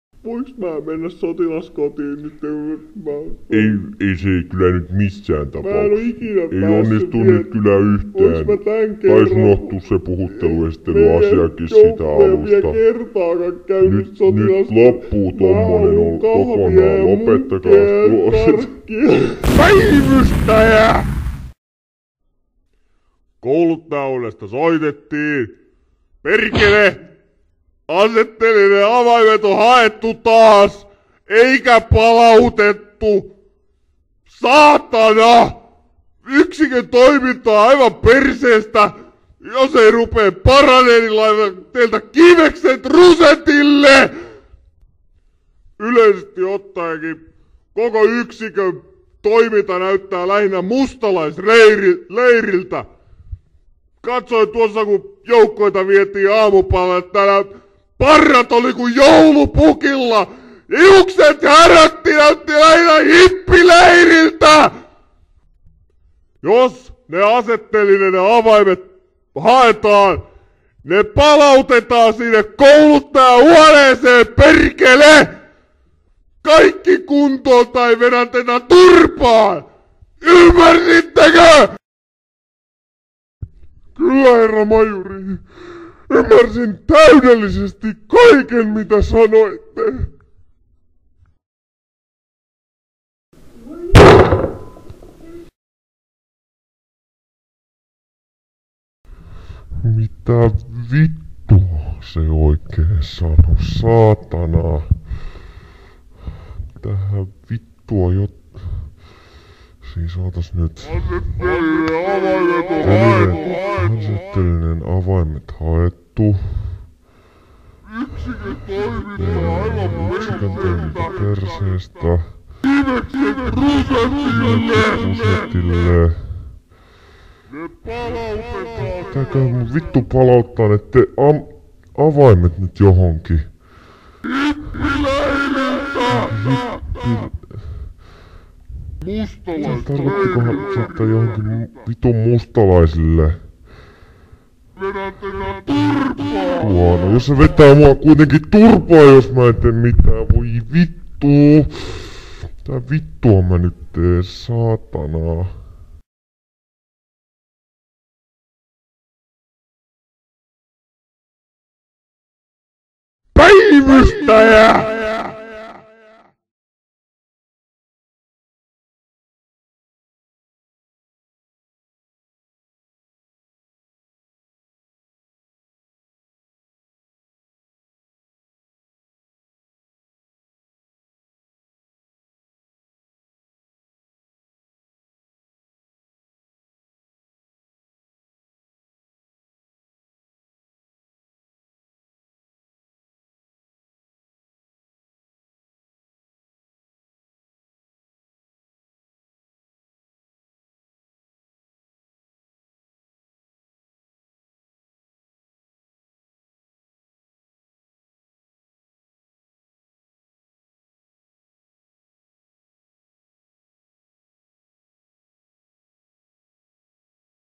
Paka 2: PÄIVYSTÄJÄ!
Jatko-osa, jossa panoksia kovennetaan ja ase-telineet palautetaan!

Tehty 2004.
Tekijät Miika Karvinen ja Ilkka Peltola

Uudelleenlataus